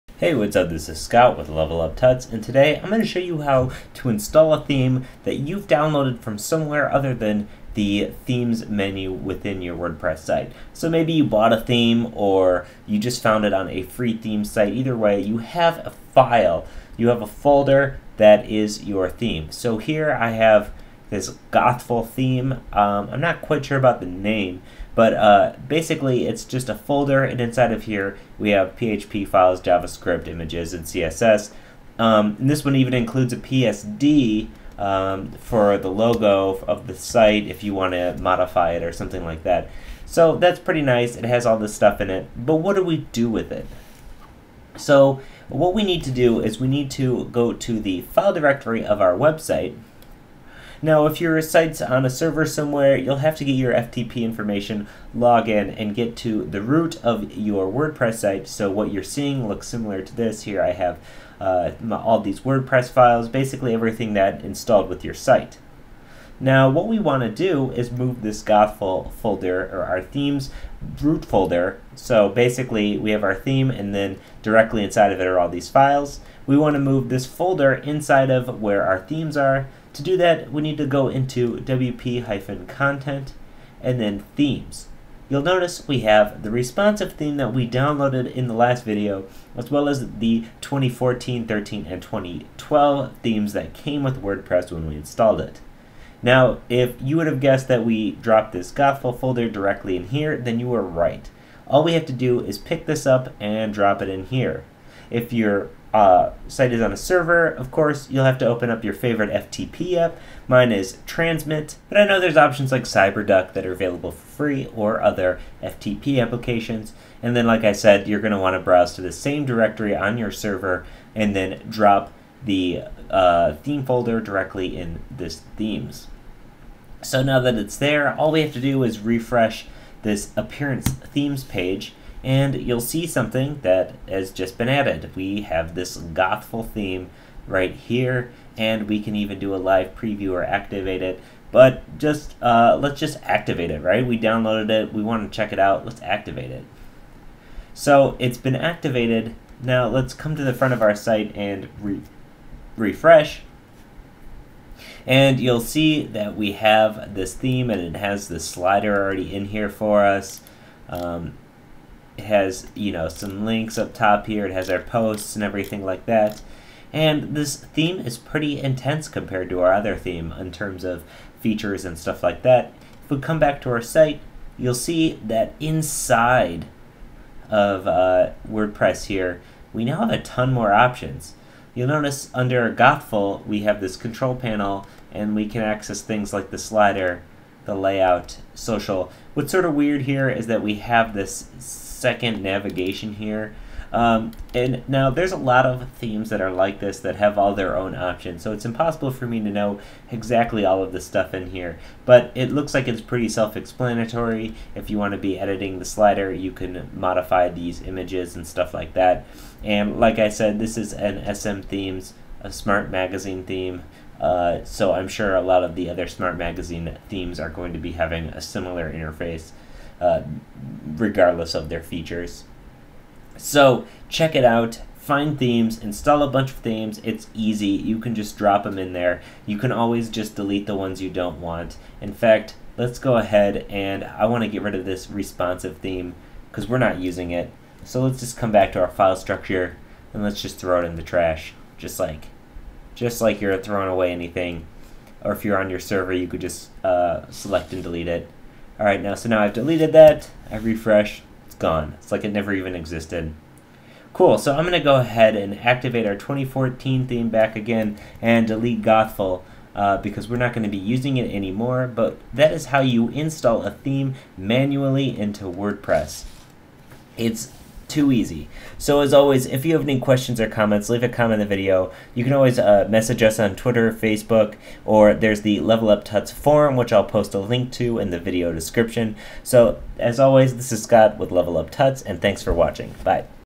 WordPress Basics #13 - Manually Adding A Theme
The 13th video in the new WordPress Basics video series. Here we show you how to install a theme manually that you have downloaded outside of WordPress.